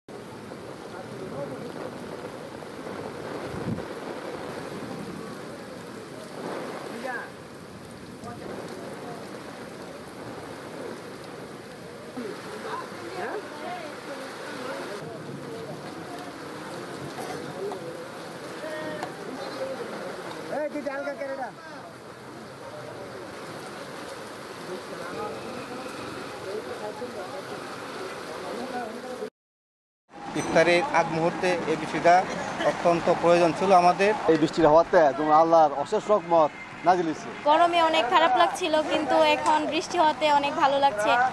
কিশোরগঞ্জের ভৈরবে বৃষ্টি হয়েছে। এতে জনজীবনে স্বস্তি নেমে আসে | Somoy TV
কিশোরগঞ্জের ভৈরবে বৃষ্টি হয়েছে। এতে জনজীবনে স্বস্তি নেমে আসে | RAIN In BD